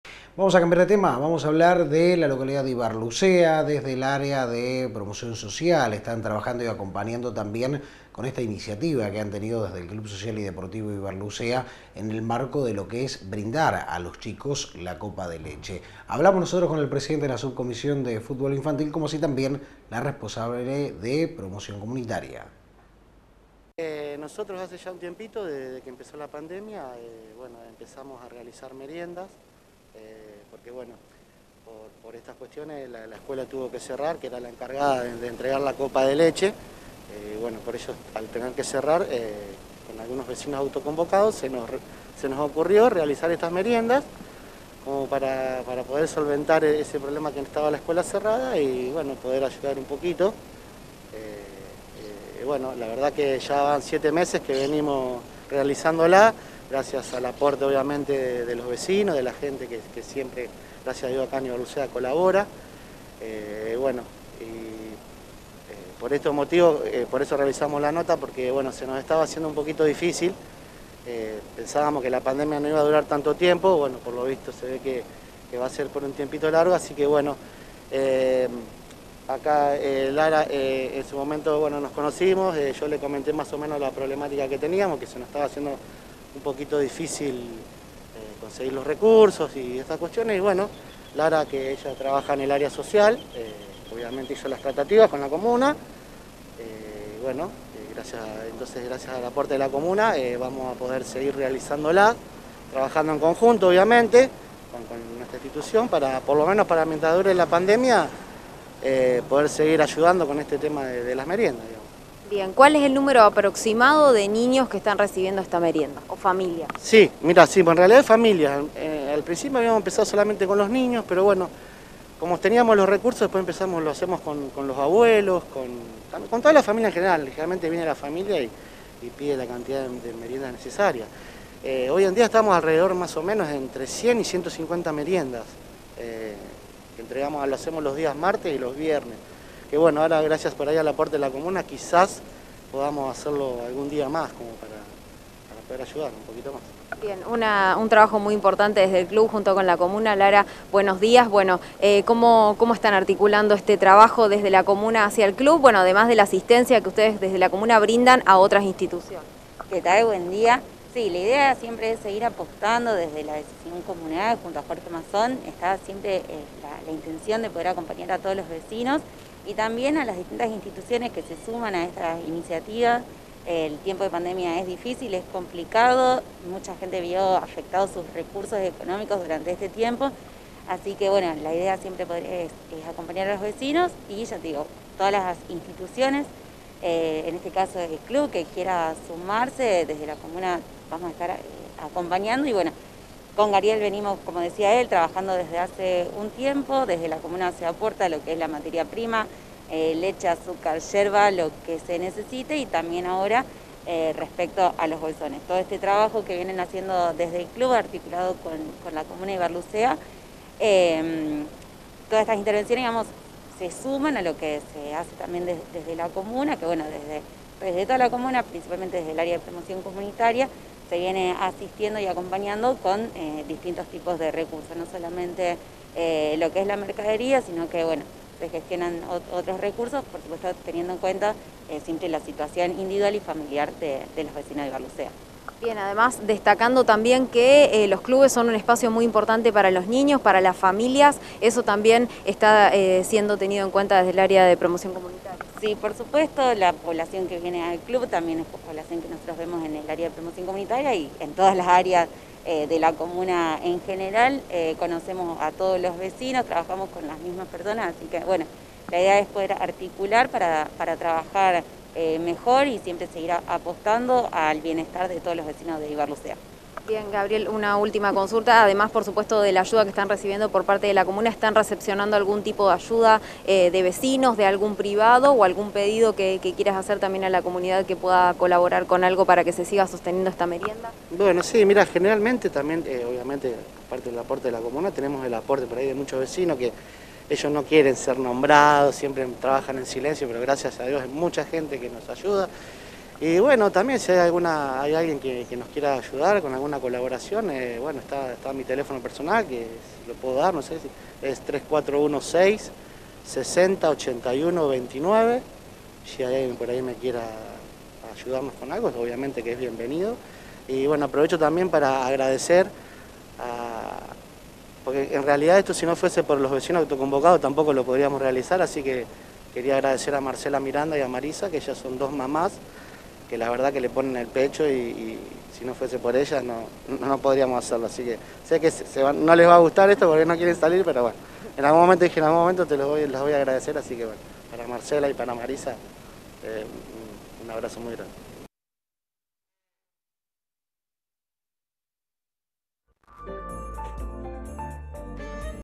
Asistencia alimentaria desde el Club Social y Deportivo Ibarlucea
El Club Social y Deportivo Ibarlucea, con la colaboración de la Comuna local, continúa coordinando la entrega de asistencia alimentaria a los más necesitados.